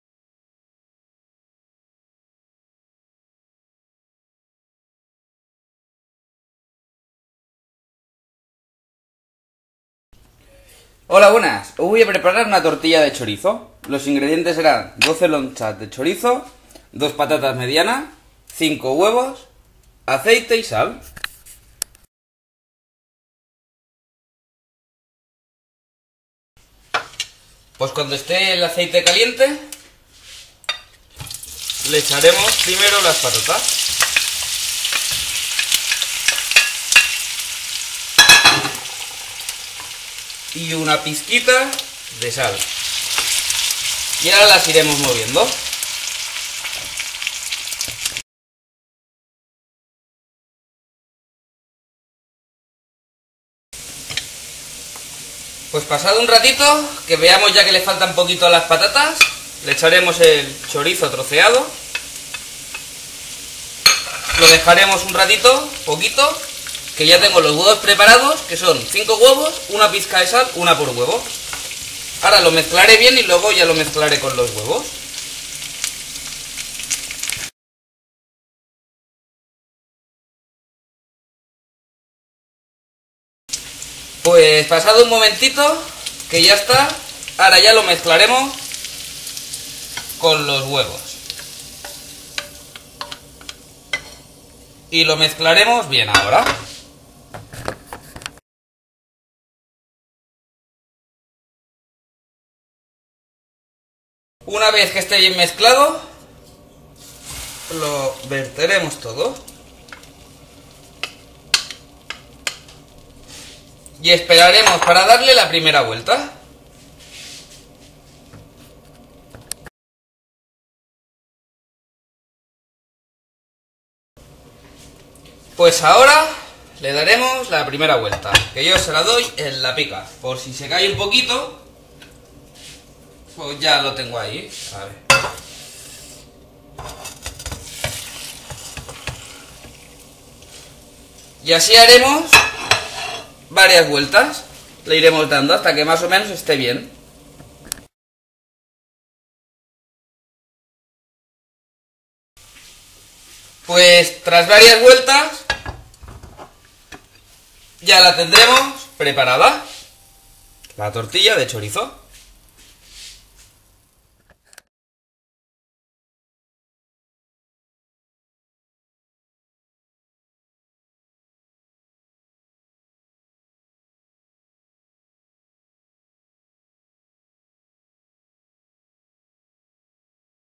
Tortilla de chorizo
Ingredientes:
2 Patatas medianas, 12 Lonchas de Chorizo, 5 huevos, Aceite y Sal